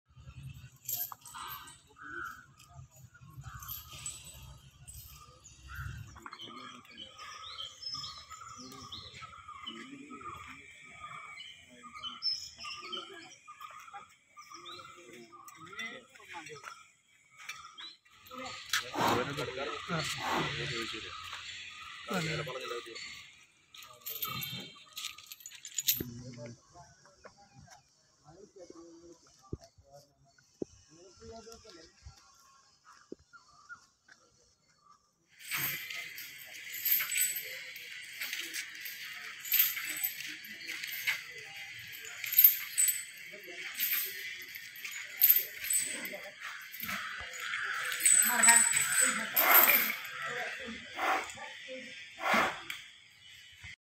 തെച്ചിക്കോട്ടുകാവിലമ്മയെ തൊഴുത് എറണാകുളം ശിവകുമാര്‍
തെച്ചിക്കോട്ടുകാവില്‍ തൊഴാന്‍ എത്തിയ എറണാകുളം ശിവകുമാര്‍